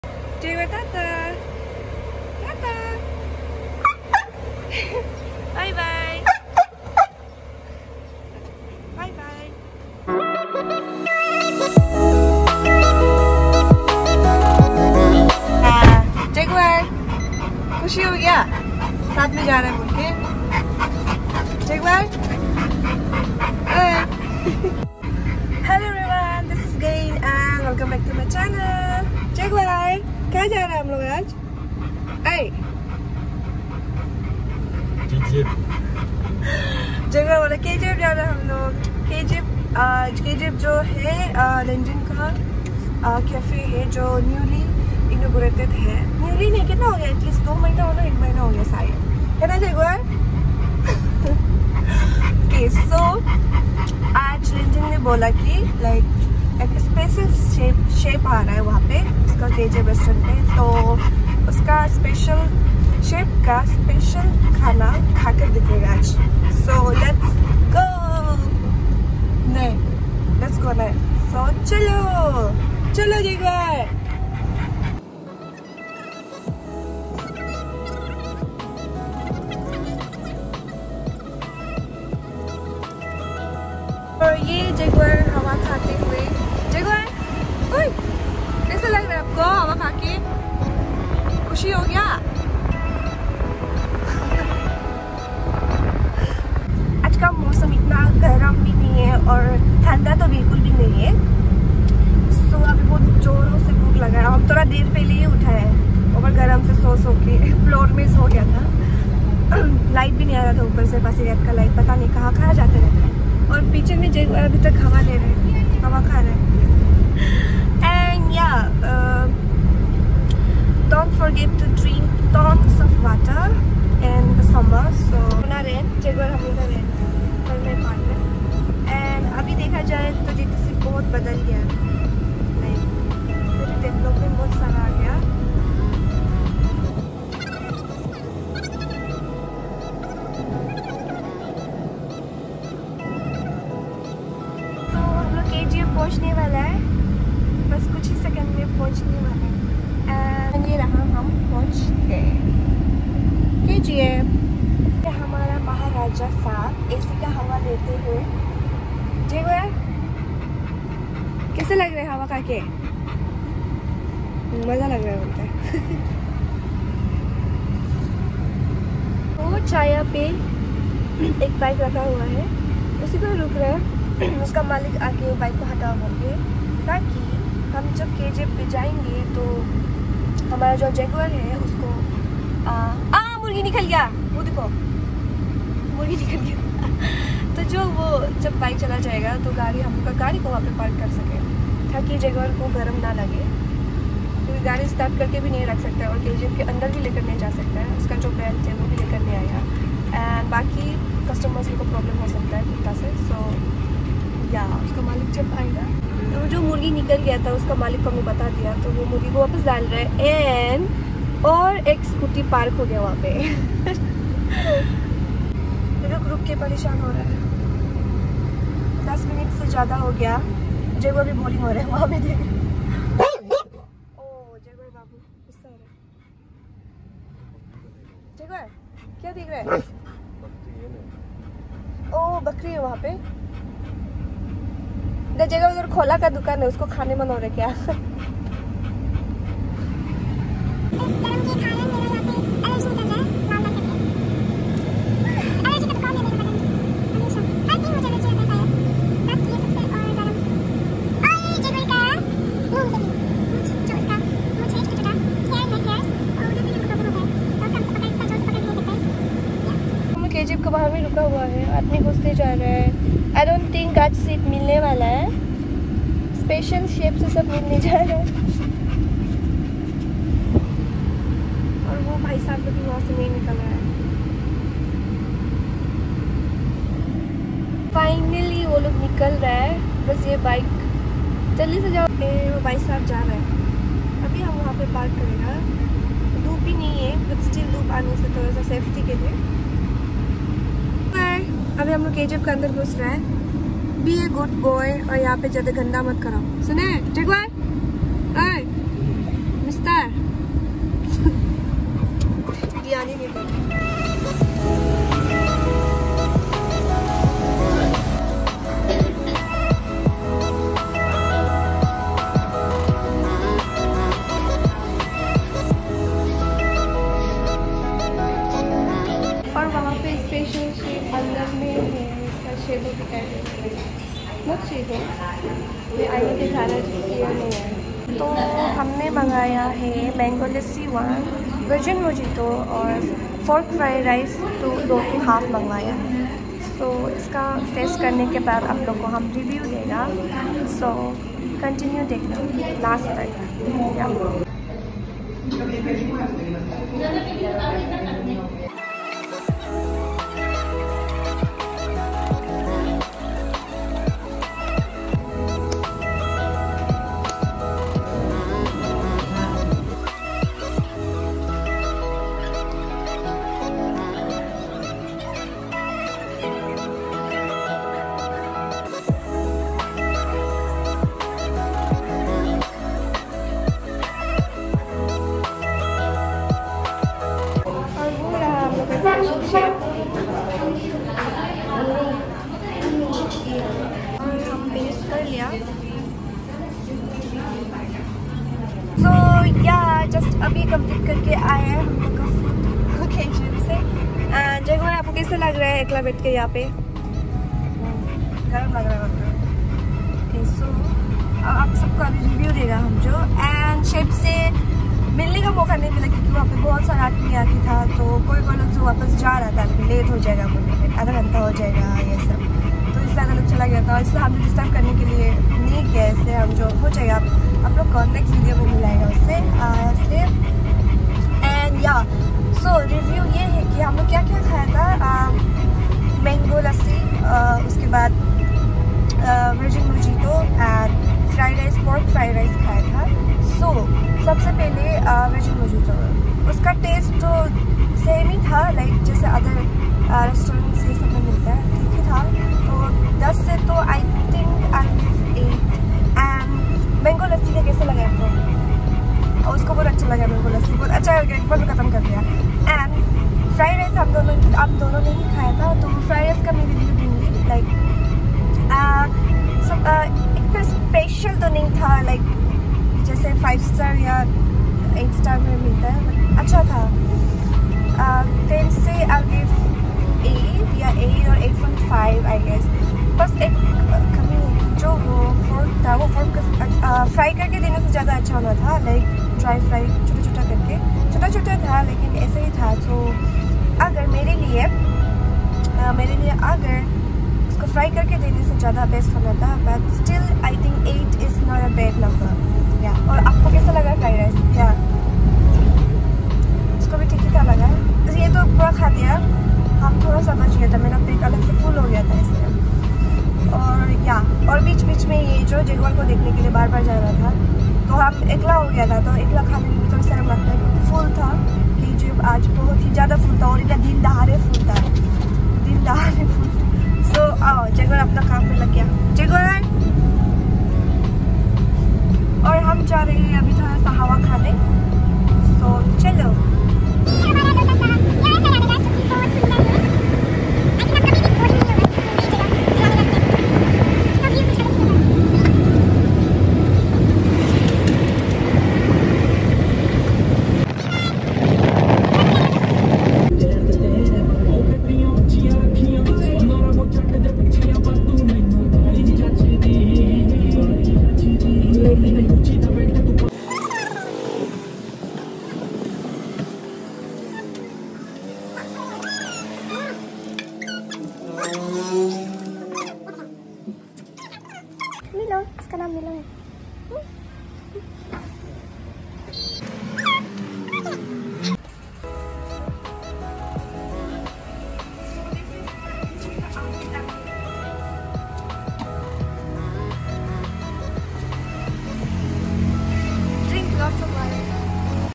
Visiting @lenzingweekly5603 restaurant || kgf || food review || meet my dog jaguar || geyin miyu
We went to @lenzingweekly5603 kgf || food review || special chef Kaun hai? || meet my dog jaguar || pasighat || Arunachal Pradesh